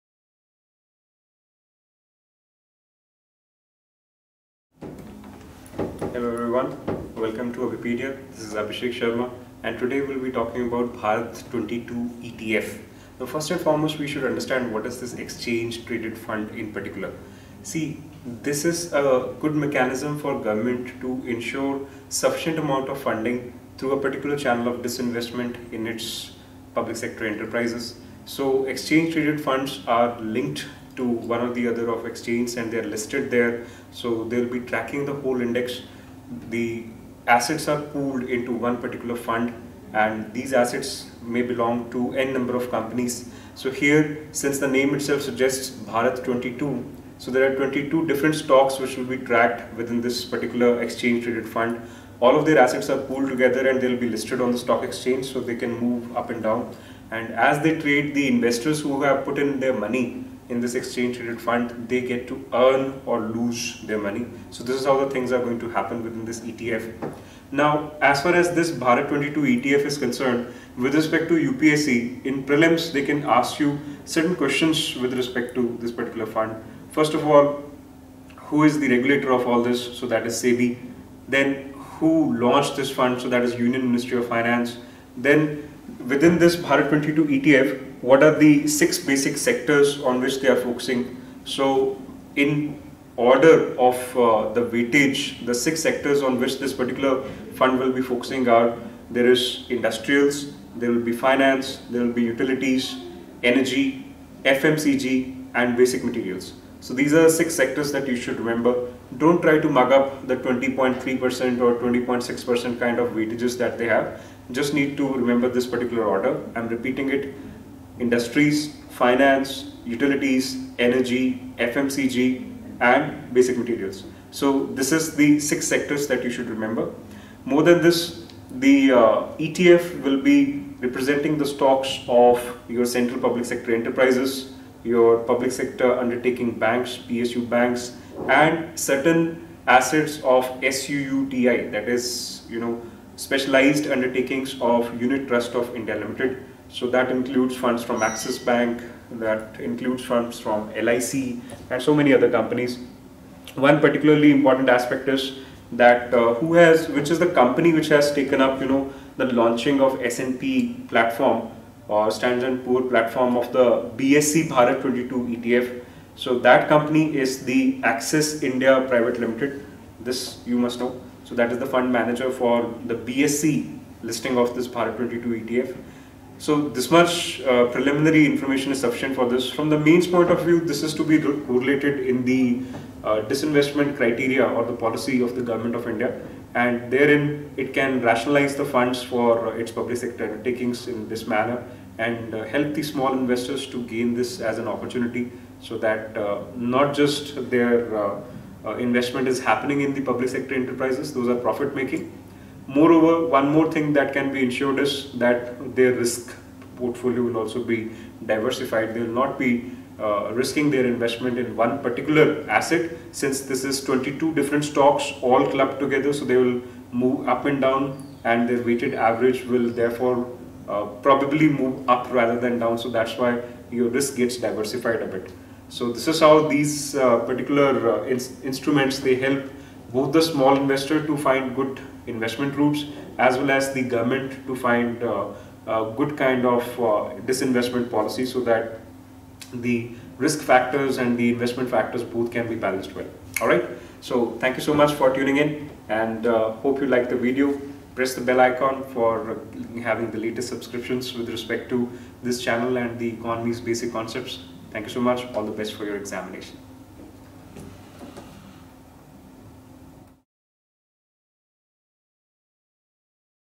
Bharat 22 ETF I Indian Economy I Basic concepts of economy I By Abhishek sir
Abhipedia powered by Abhimanu IAS brings a "Basic concepts of economy"to sharpen your preparation for IAS 2020.